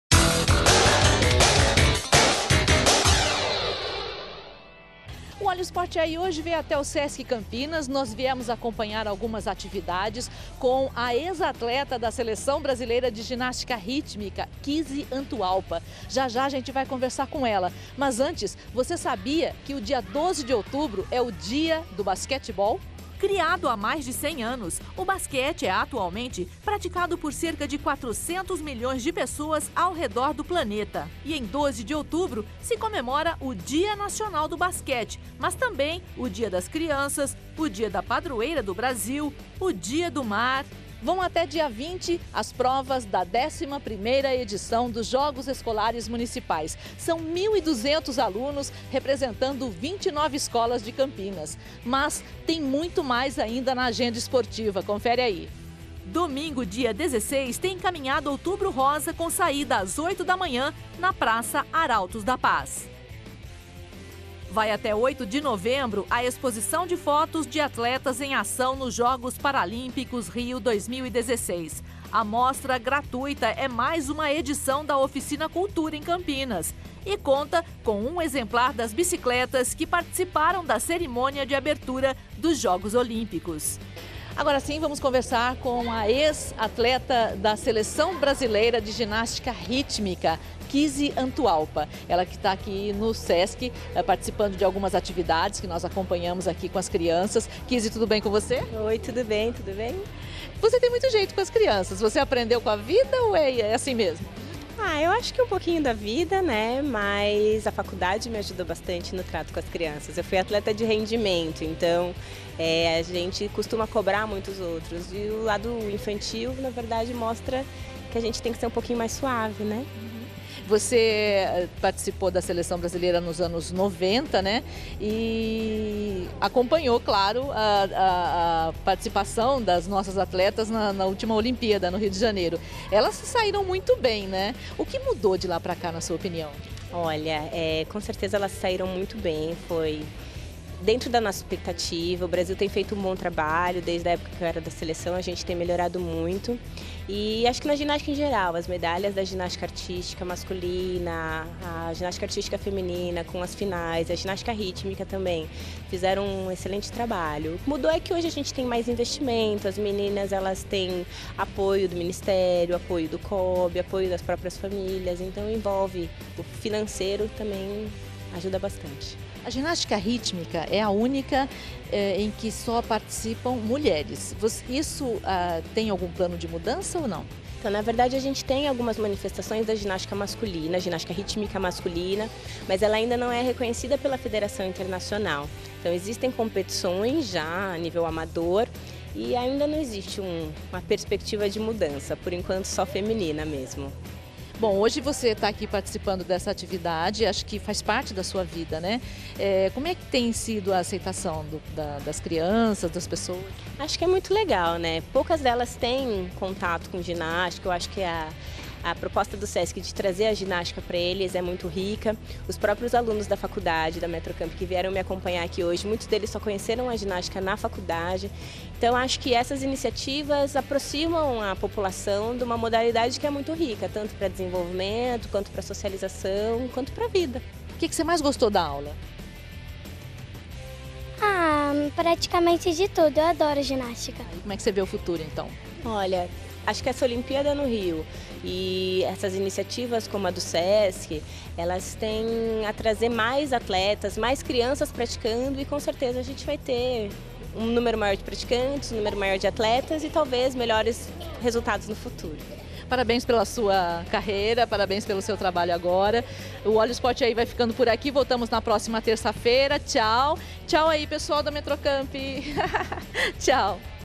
Câmara Notícia : Olha o esporte aí 11/10/2016
Dia do basquete se comemora em 12 de outubro. Jogos escolares municipais vão até dia 20. Dia 16 tem Caminhada Outubro Rosa. Bicicleta usada na abertura dos Jogos Olímpicos Rio 2016 está exposta em Campinas até 8 de novembro. Kizzy Antualpa, ex-atleta da seleção brasileira de ginástica rítmica esteve no Sesc de Campinas e conversou com a repórter Ana Bresil.